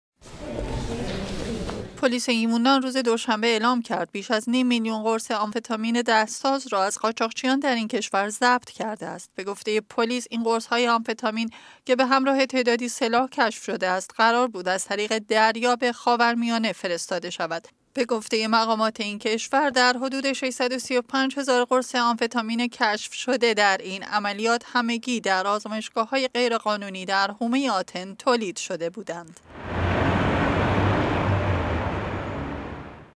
کشف بیش از نیم میلیون قرص آمفتامین در یونان
* خبر کوتاه *

پلیس یونان روز دوشنبه اعلام کرد بیش از نیم میلیون قرص آمفتامین دست ساز را از قاچاقچیان در این کشور ضبط کرده است. به گفتۀ پلیس یونان، این قرص های آمفتامین که بهمراه تعدادی سلاح کشف شده است، قرار بود از طریق دریا به خاورمیانه فرستاده شود.

عملیات کشف این شبکۀ قاچاق که از شش ماه پیش آغاز شده با همکاری پلیس یونان و ادارۀ جرائم مالی این کشور صورت گرفته است.

پلیس یونان به نقل از قاچاقچیان دستگیر شده می گوید در حدود ۶۳۵۰۰۰ قرص آمفتامین که در آزمایشگاههای غیرقانونی در حومۀ آتن ساخته شده …